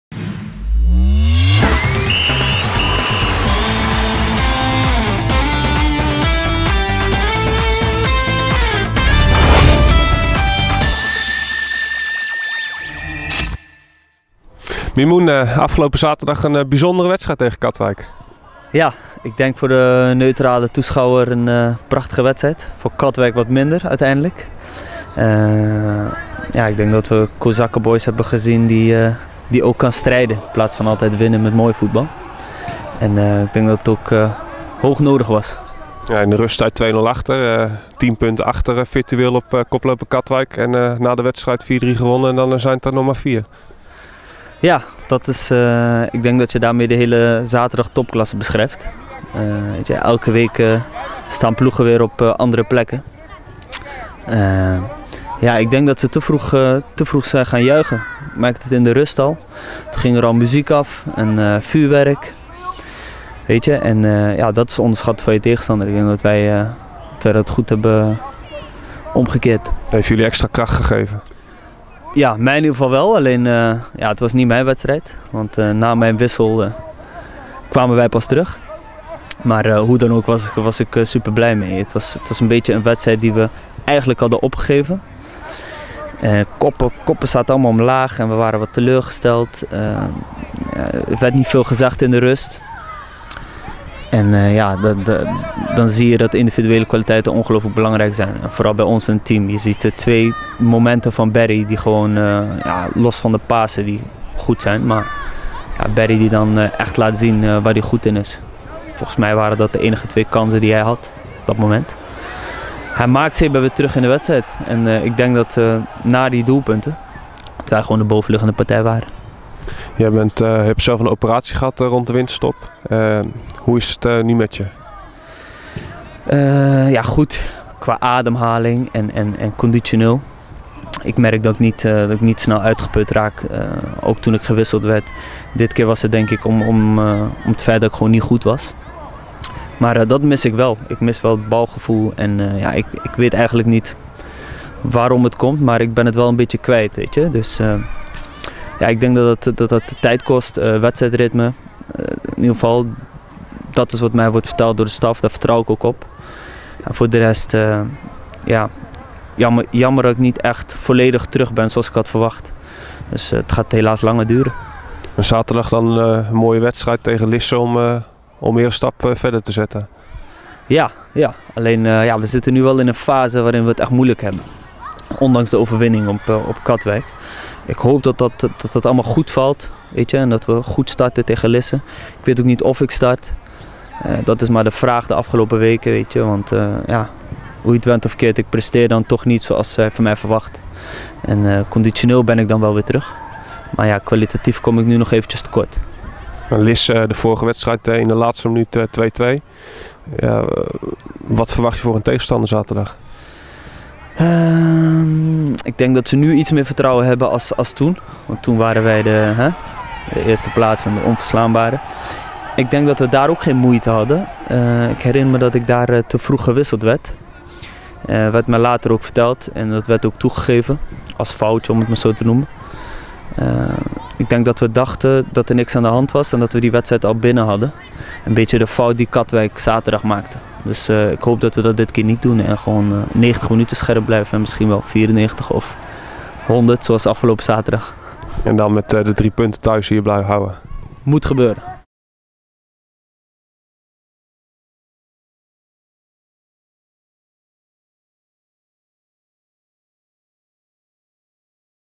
Voorbeschouwing Kozakken Boys - FC Lisse 15/16
In aanloop naar de topklassewedstrijd Kozakken Boys - FC Lisse van zaterdag 19 maart sprak Kozakken Boys TV met Mimoun Eloisghiri.